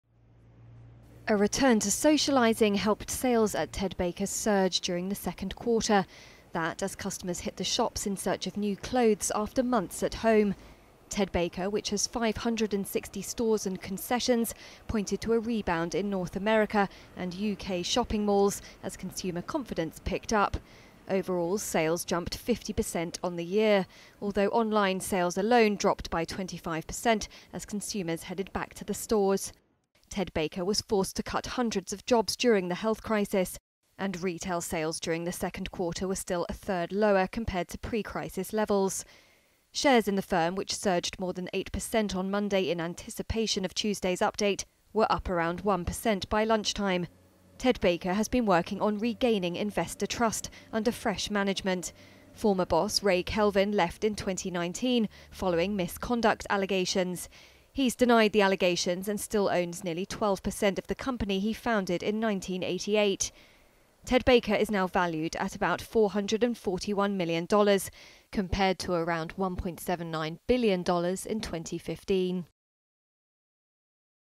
Ted Baker enjoys surge as dressing up returns
Fashion retailer Ted Baker's sales surged during the second quarter as customers returned to shops after months of stay-at-home orders in search of new clothes for socializing again.